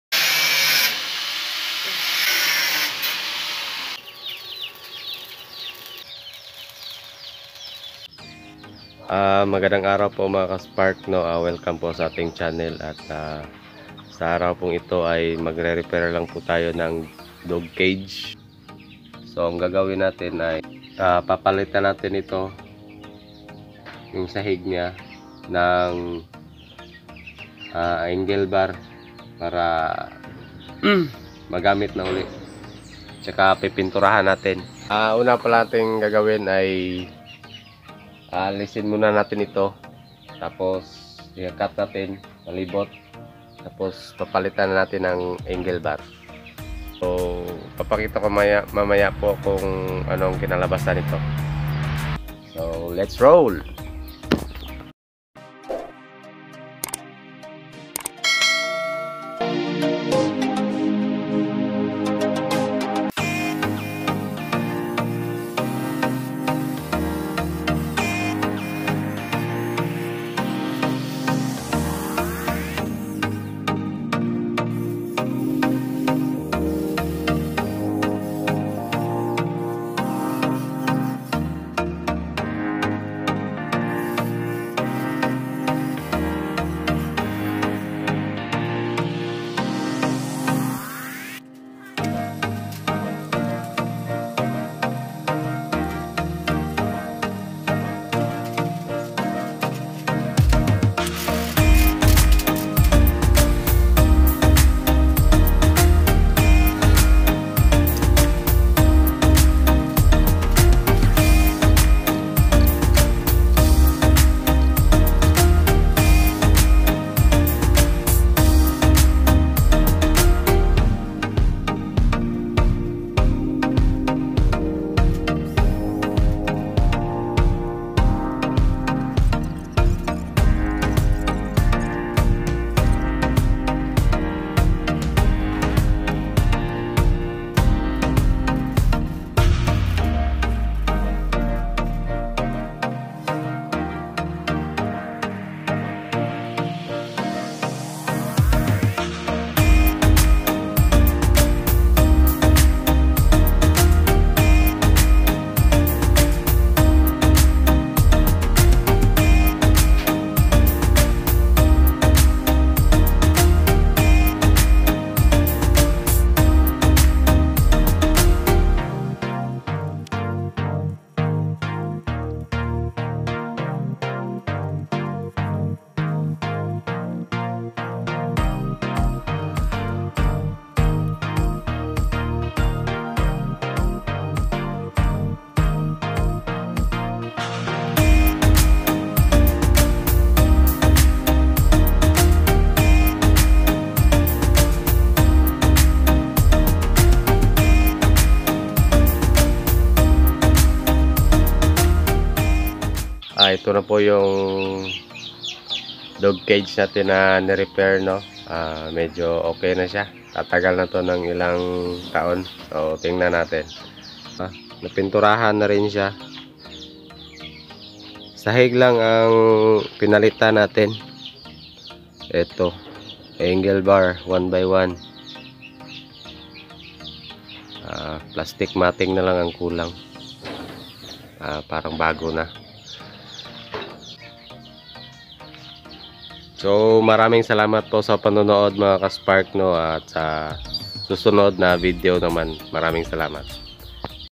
Nirepair ko ang bulok na DOG CAGE | DOG CAGE REPAIR
Magandang araw po mga Kaspark! sa videong ito ay mag rerepair tayo ng dog cage angle bar 1×1 lng ang aking ginamit para palitan ang nabulok na flooring .. sana'y inyong magustohan at makunan ng konting ideya .. at kung nagustohan mo ang video na ito ay please like, share, comments and SUBSCRIBE at iclick mo na rin ang bell button kung bago ka pa lang sa aking channel para updated ka sa aking susunod na videos! maraming salamat!